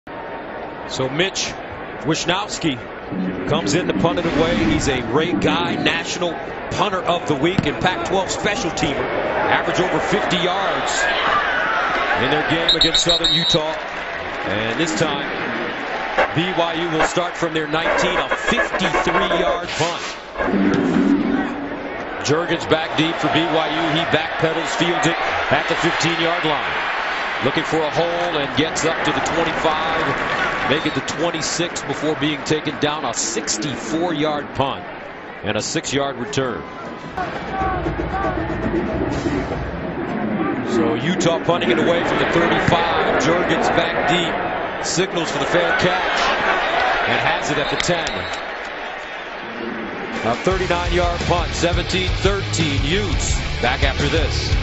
2016 Utah vs. BYU - Mitch Wishnowsky
University of Utah vs. BYU

Mitch Wishnowsky punt for 53 yds
Mitch Wishnowsky punt for 65 yds , Garrett Juergens returns for 9 yds to the BYU 26
Mitch Wishnowsky punt for 39 yds, fair catch by Garrett Juergens at the BYU 11